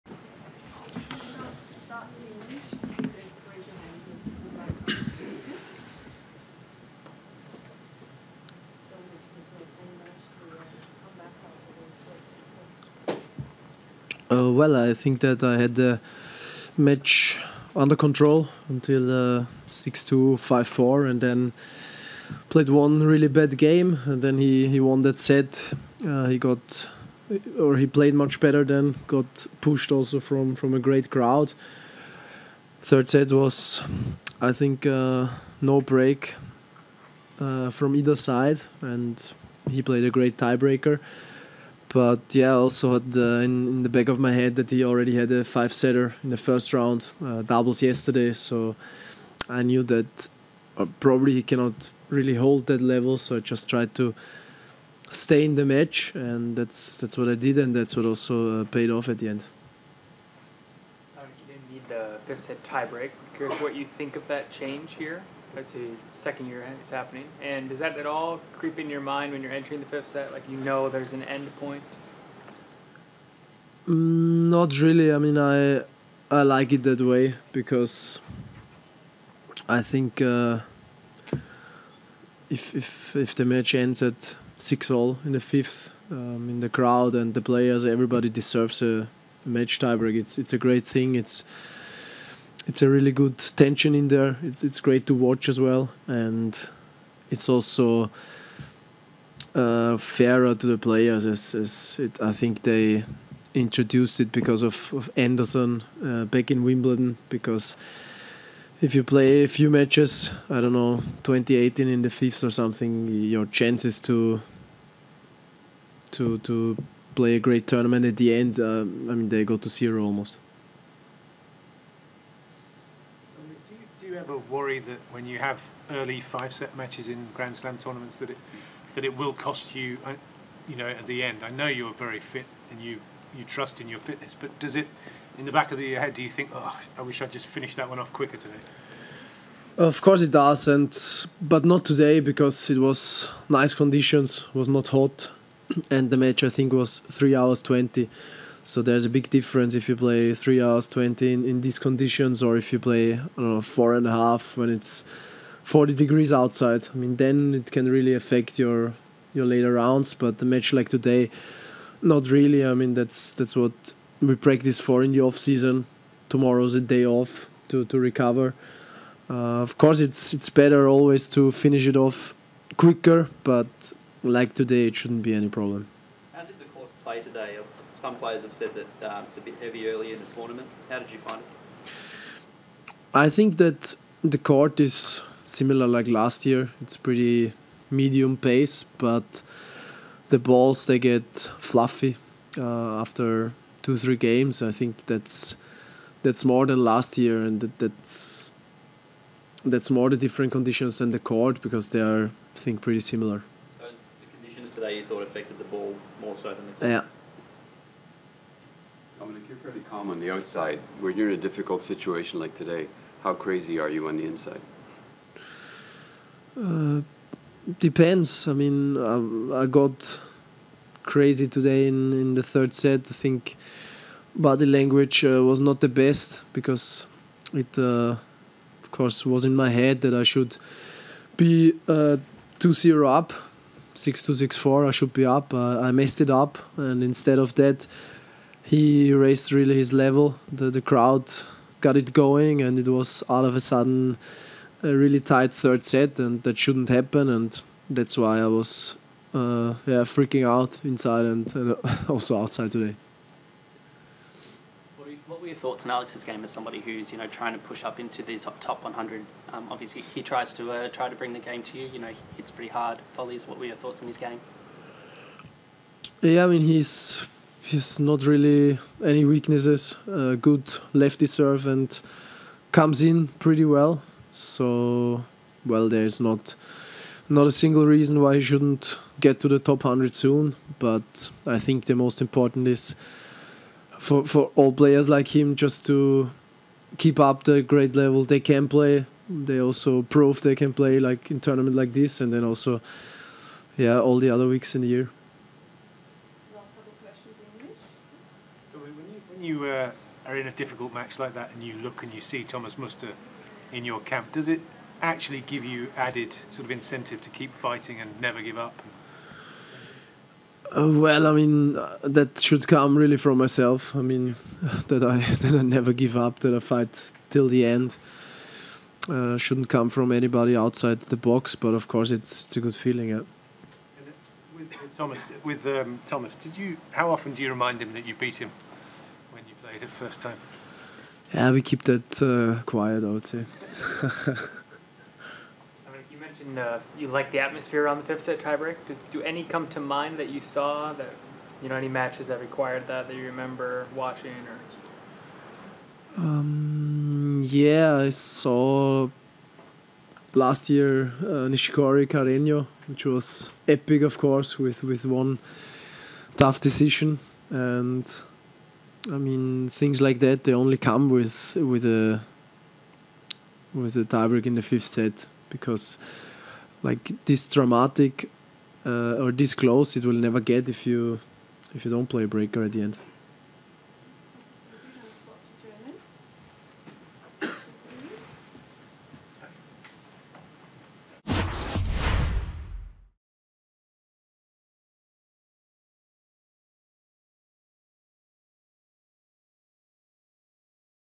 Dominic Thiem: "I had the match under control" | Australian Open 2020 Press Conference 2R
Press conference with Dominic Thiem in the second round of the Australian Open 2020.